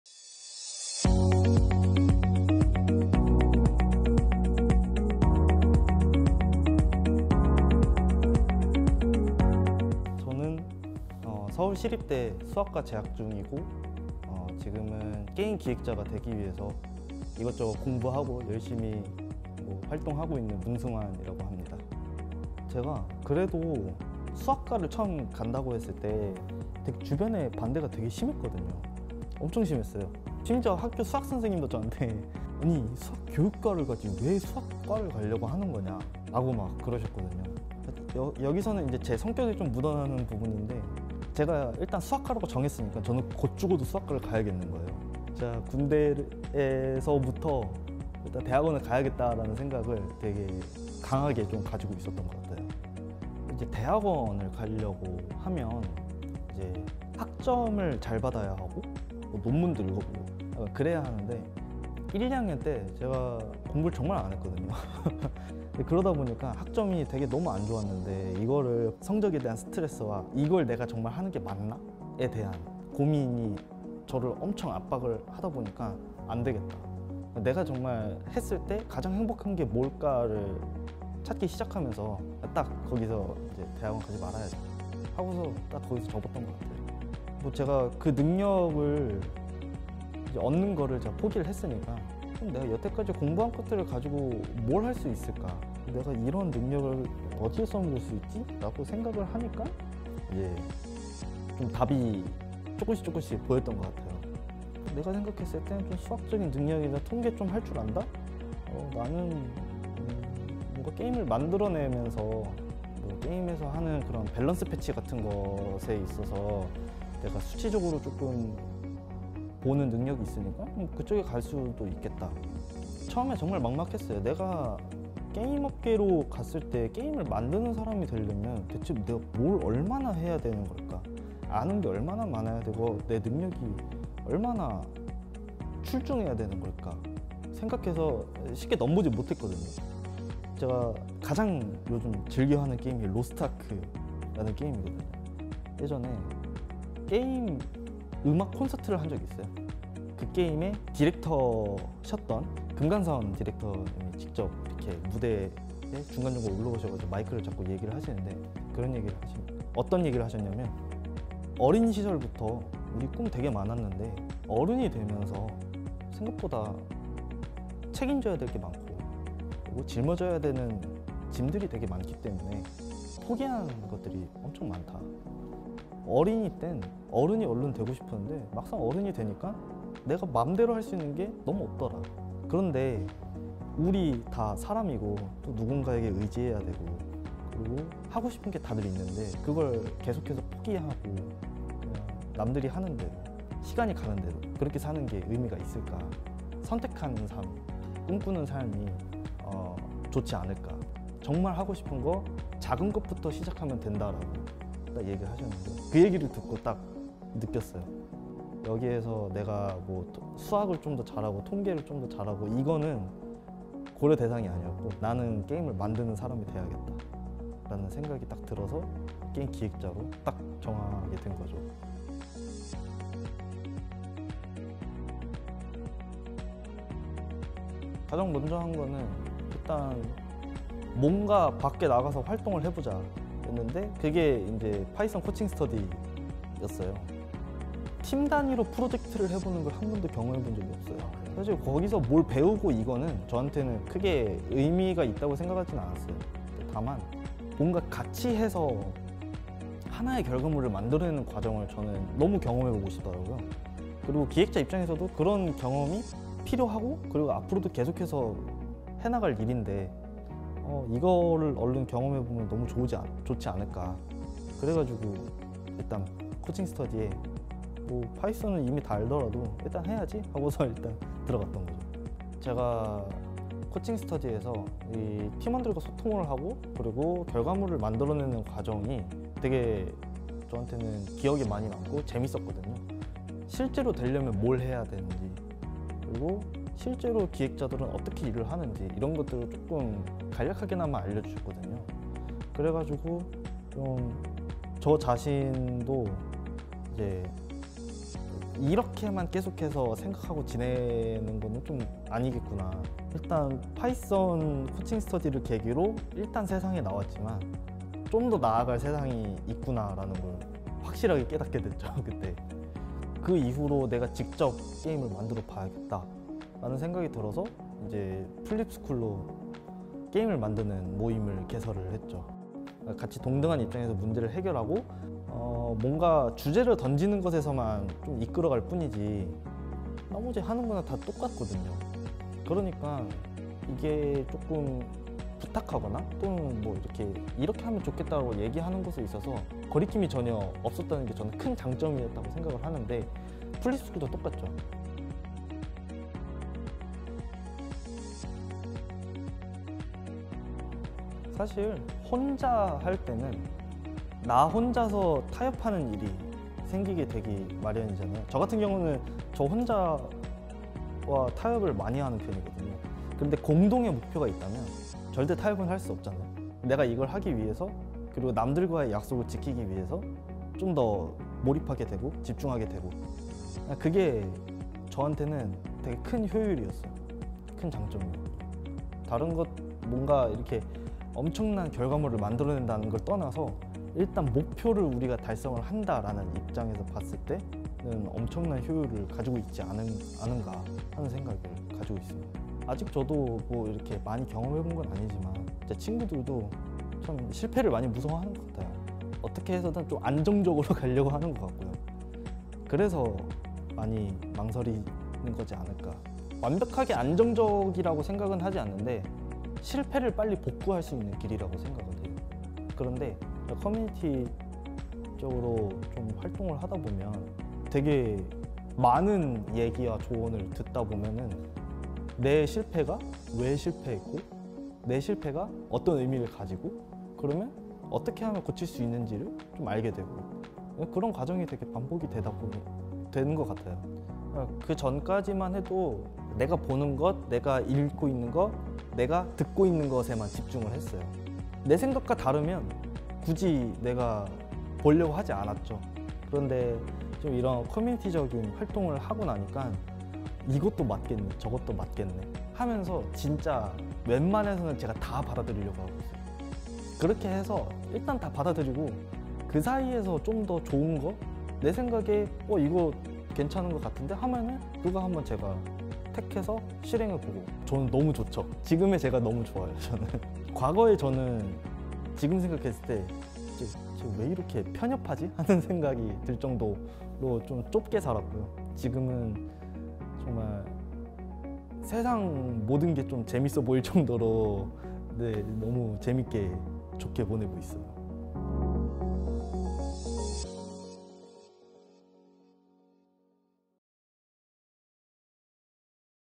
수학 전공에서 게임 기획자로 진로를 바꾸게 된 이야기 - 문승환 I 커뮤니티를 묻다
모두가 지친 평일 저녁에 하나, 둘 모이기 시작하는 엉뚱한 연구소가 있다? 🧐
함께 성장하는 커뮤니티 모두의연구소의 진솔한 이야기🌱 - 문승환님편

이번 주인공은 수학 전공에서 게임 기획자로 진로를 바꾸게 된 문승환님이에요.
코칭스터디에서 풀잎스쿨까지, 커뮤니티를 통해 성장하고 있는 이야기를 지금 영상으로 만나보세요!